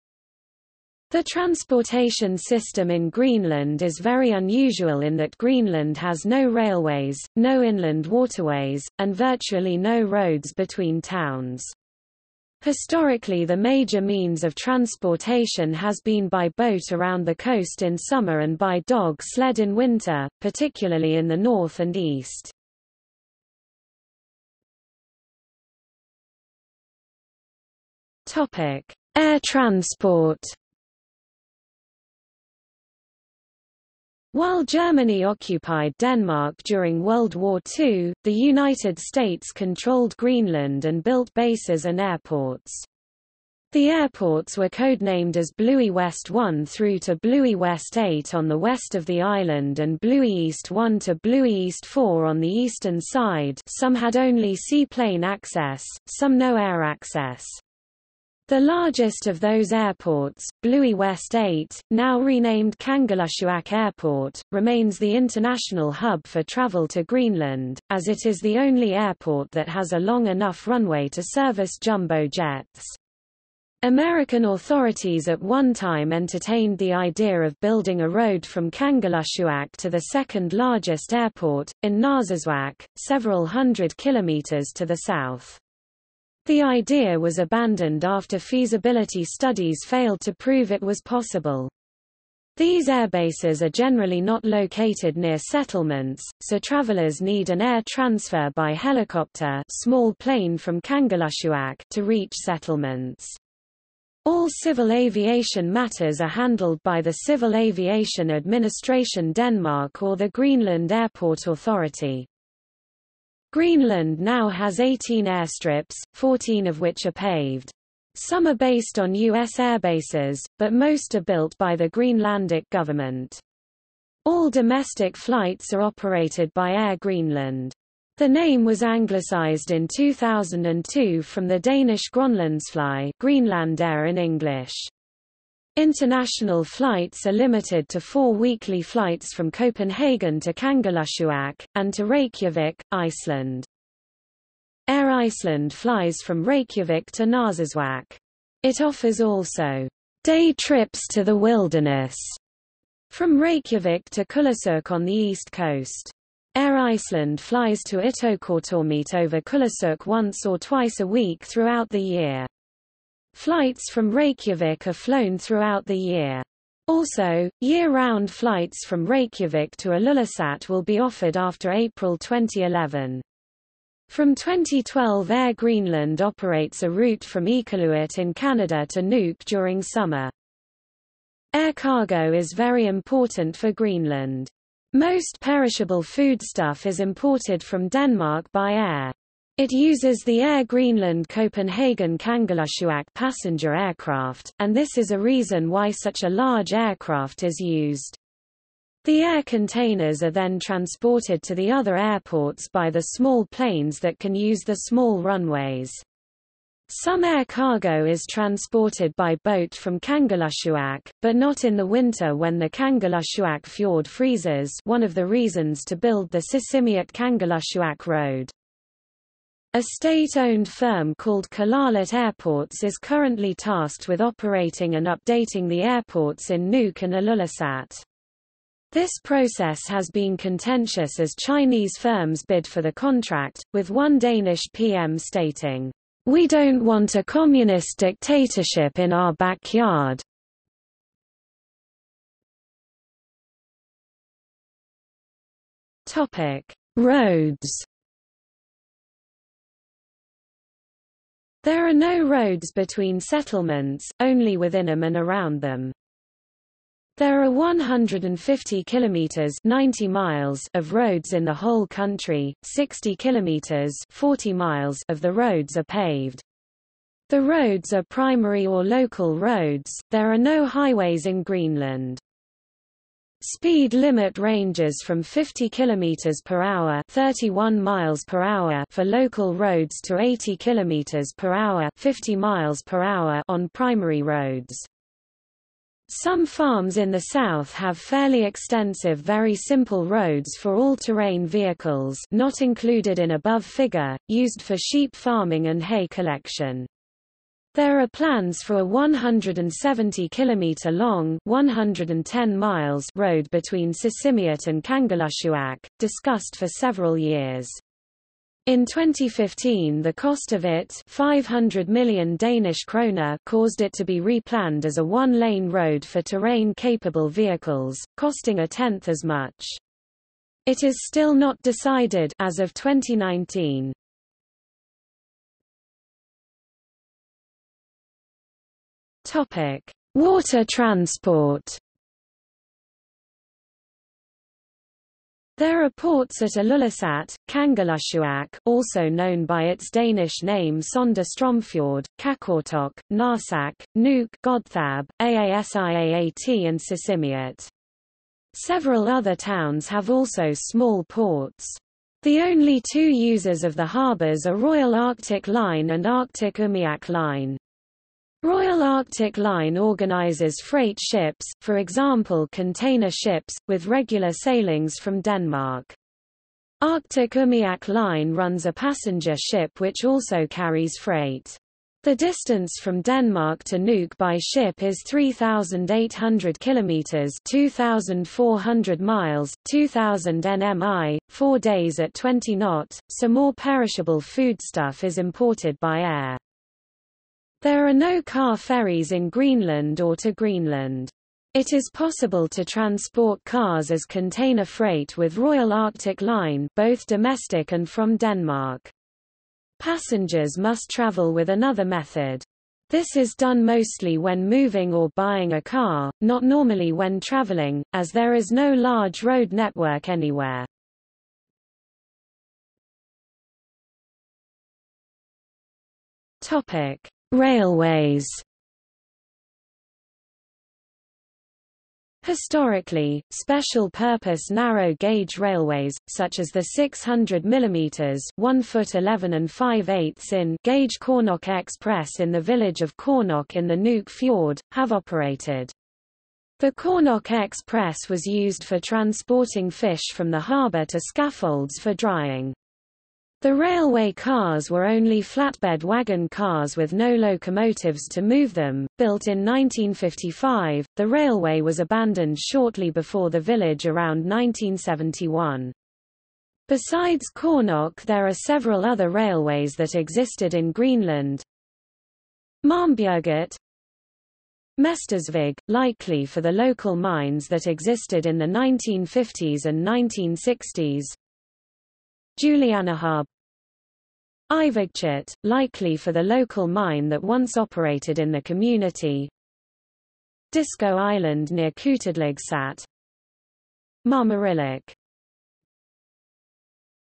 Transportation in Greenland | Wikipedia audio article
00:00:24 1 Air transport
00:04:02 2 Roads
00:05:25 3 Water transport
00:06:52 4 Railways

- Socrates

SUMMARY
The transportation system in Greenland is very unusual in that Greenland has no railways, no inland waterways, and virtually no roads between towns. Historically the major means of transportation has been by boat around the coast in summer and by dog sled in winter, particularly in the north and east.